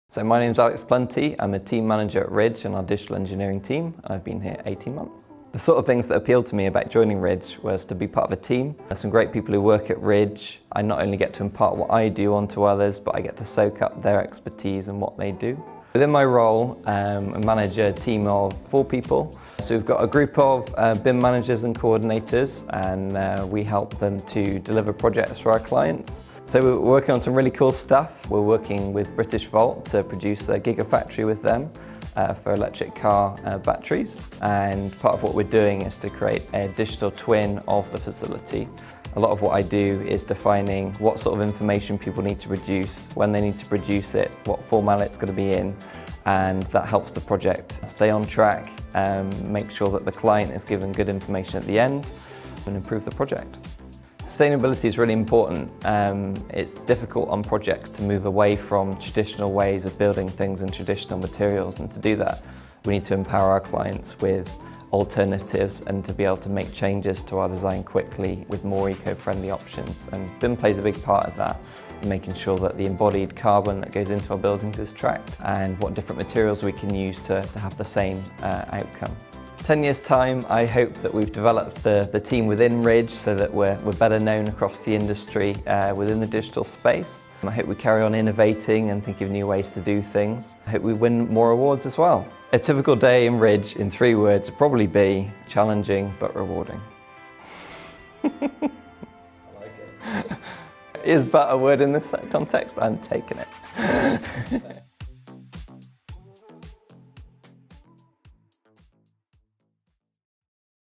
Careers at Ridge - Alex, BIM Manager
Alex, BIM Manager, tells us about his role at Ridge, specialising in Building Information Modelling (BIM) and Digital Engineering. Ridge is an award-winning multidiscipline property and construction consultancy.